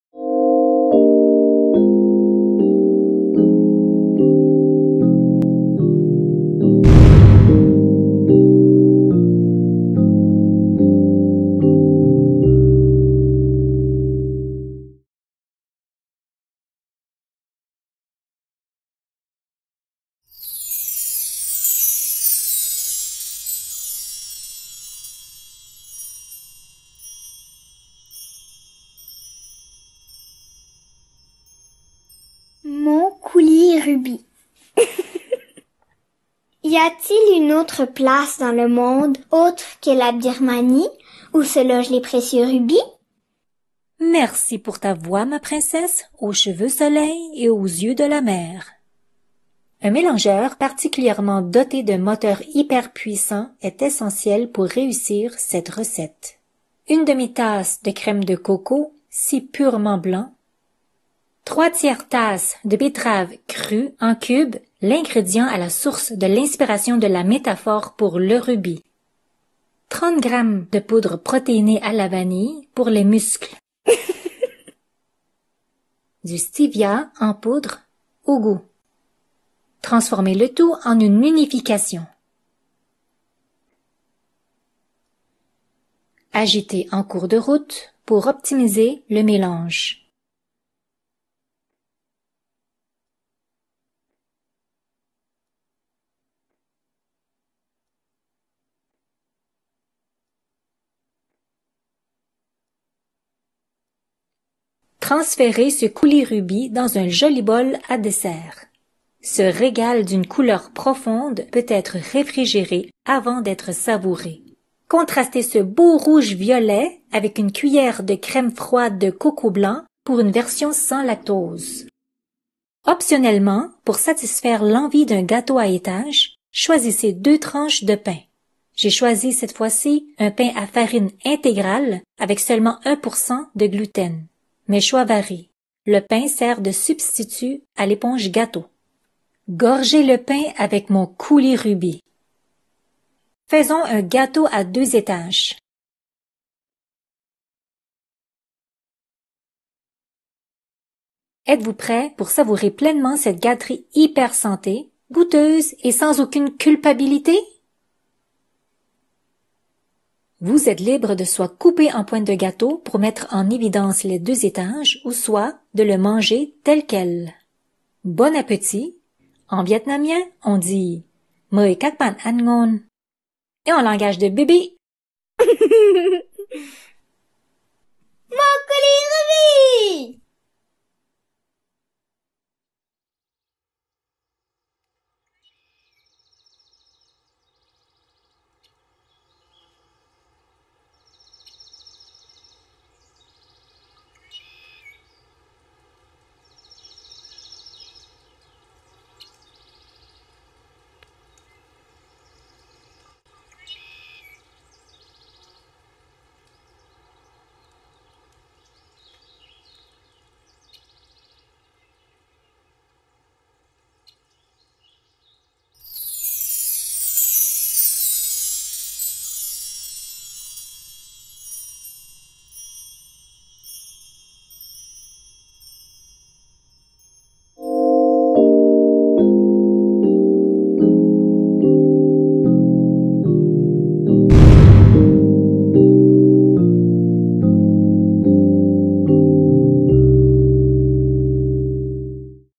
Coulis-Rubis, avec Thuy Hang
Ce coulis-rubis est sucré tout en assurant un index glycémique bas et est très rapide à faire. Recette tirée du livre La cuisine d’inspiration vietnamienne de Thuy Hang

Voici les quelques extraits du livre: C’est en Birmanie que se trouvent les rubis les plus précieux. C’est la fameuse betterave qui donne cette belle couleur au coulis-rubis.

Le coulis-rubis m’inspire. L’idée que ce coulis-rubis contient des antioxydants, des fibres, des vitamines et des minéraux est réjouissante…

La betterave, à partir de laquelle il est fait, a un goût sucré, mais une teneur faible en glucides (un index glycémique bas), et sa couleur vivifiante comble les sens. Elle joue un rôle dans la stabilité de la glycémie.

La préparation de ce coulis-rubis, requiert très peu de temps et d’outils.
Il existe plusieurs façons de déguster ce coulis-rubis.

Avec quelques modifications dans les ingrédients, il peut aussi bien devenir un repas sur le pouce qu’une collation, une sauce d’accompagnement ou un coulis pour verser sur un gâteau, une tarte... ou un dessert.

Le tout expliqué dans le livre.

14F